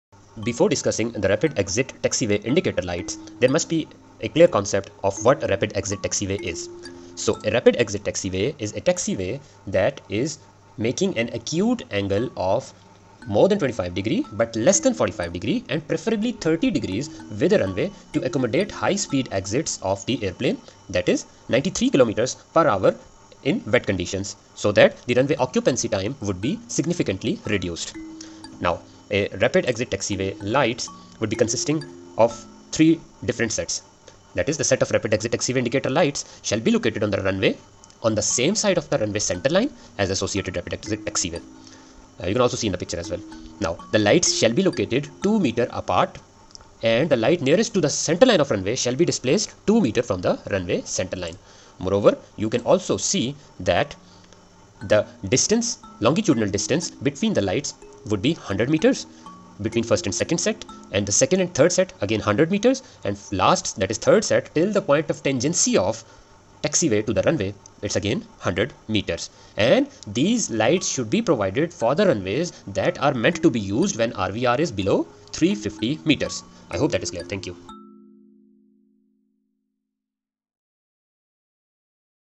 Rapid Exit Taxiway Indicator Lights Explained | AFLS | ICAO | Annex 14 | The World of ATC
Rapid exit taxiway indicator lights shall be fixed unidirectional yellow lights, aligned so as to be visible to the pilot of a landing aeroplane in the direction of approach to the runway.
The purpose of rapid exit taxiway indicator lights (RETILs) is to provide pilots with distance-to-go information  to the nearest rapid exit taxiway on the runway, to enhance situational awareness in low visibility conditions and enable pilots to apply braking action for more efficient roll-out and runway exit speeds. It is essential that pilots operating at aerodromes with runway(s) displaying rapid exit taxiway indicator lights be familiar with the purpose of these lights.
A set of rapid exit taxiway indicator lights shall be located on the runway on the same side of the runway centre line as the associated rapid exit taxiway
In each set, the lights shall be located 2 m apart and the light nearest to the runway centre line shall be displaced 2 m from the runway centre line